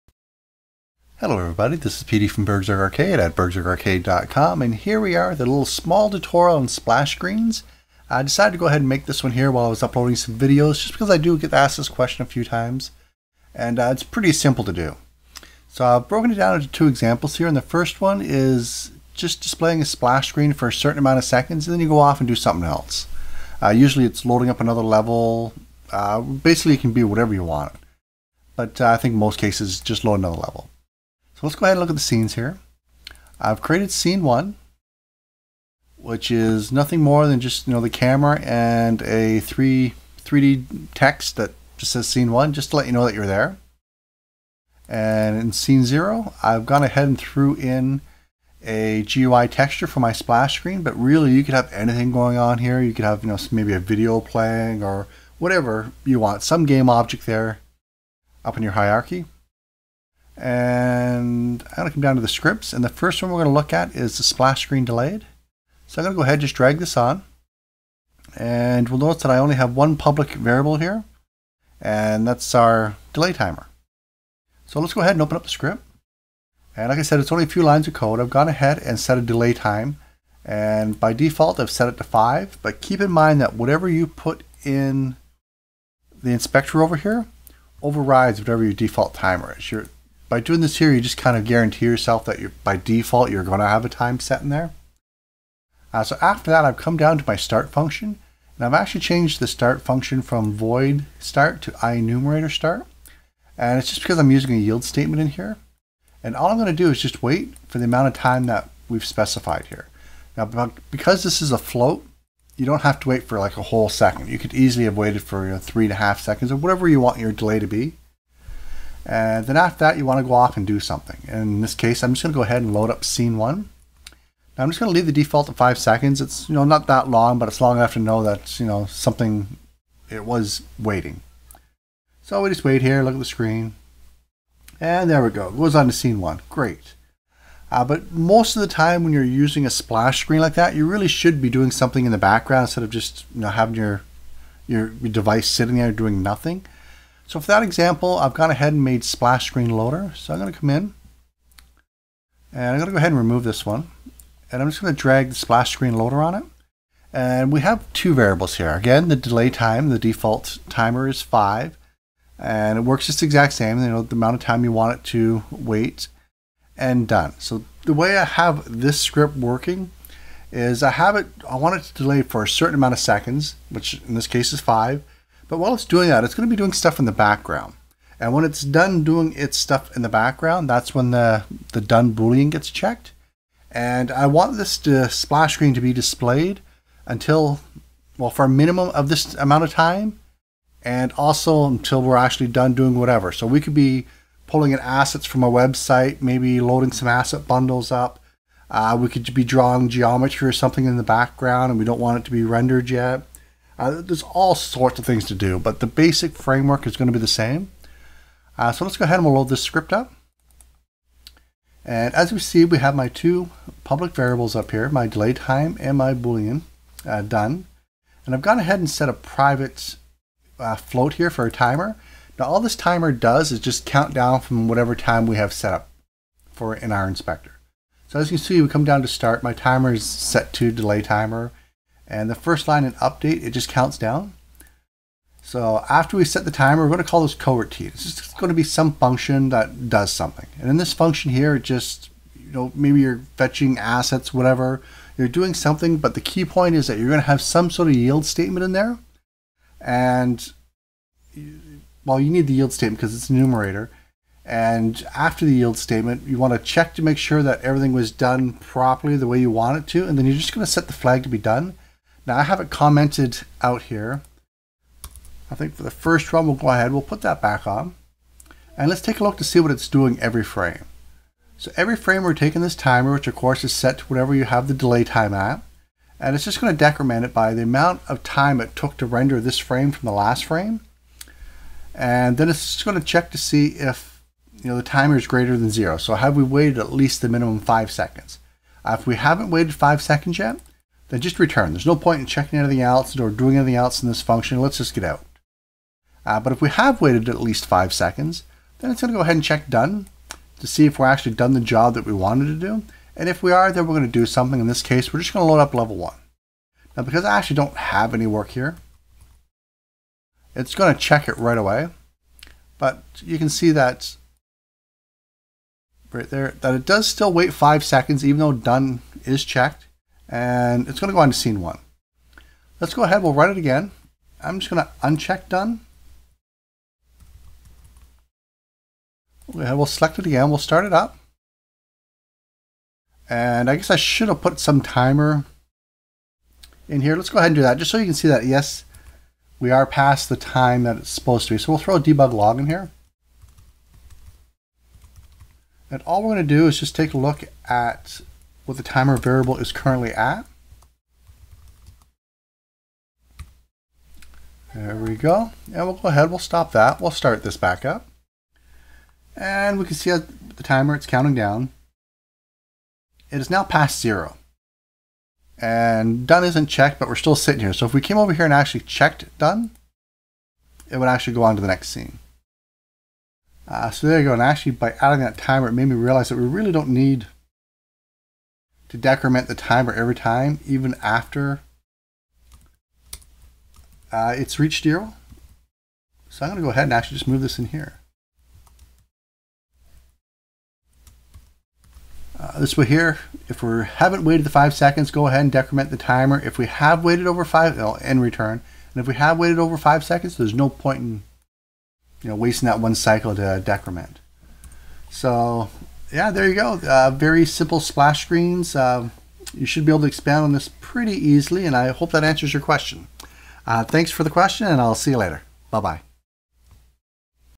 Unity3d Splash Screens
Here is a small tutorial on a couple of ways to create a Splash Screen in Unity3d.